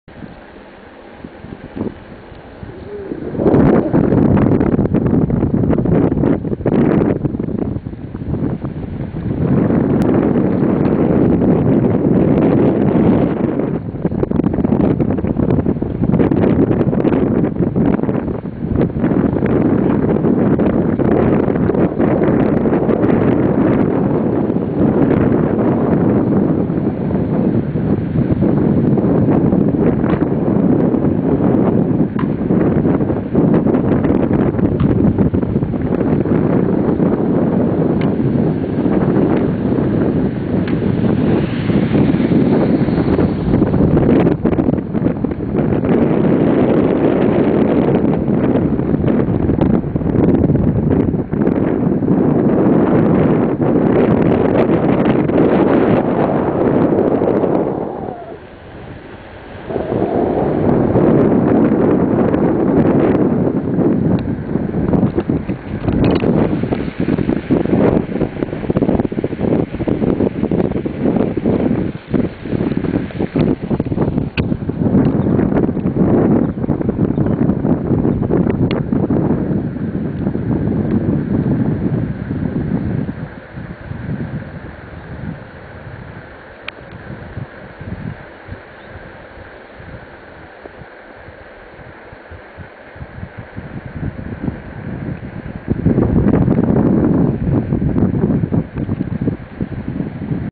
bike.traveling
JASTRZĘBIK ...zjazd z Jaworzyny w kierunku Muszyny
09.2005